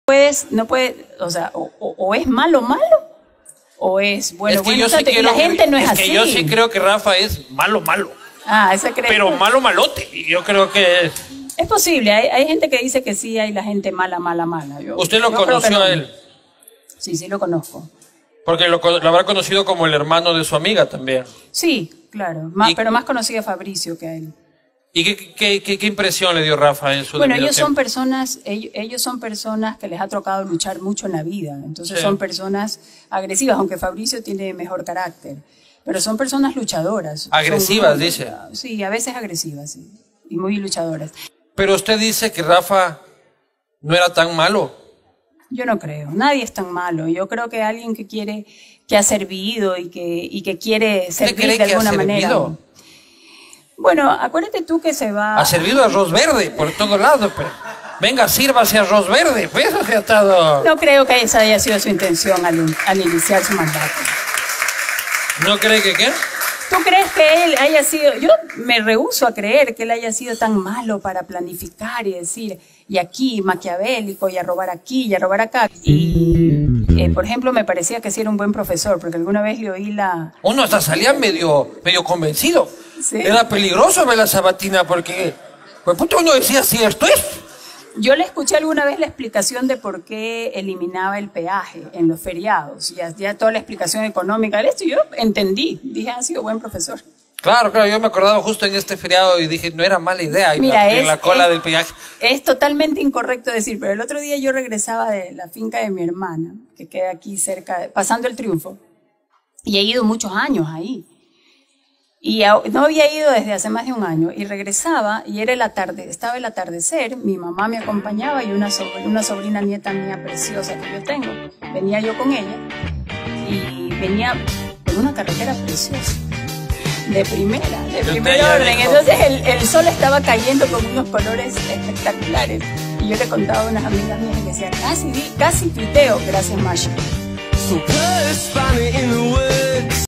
Periodista María Teresa Arboleda de Shotel, reconoce obras del gobierno del ex presidente Correa
VideoBananero| Periodista María Teresa Arboleda de Shotel, reconoce obras del gobierno del ex presidente Rafael Correa Delgado, durante entrevista de La Posta.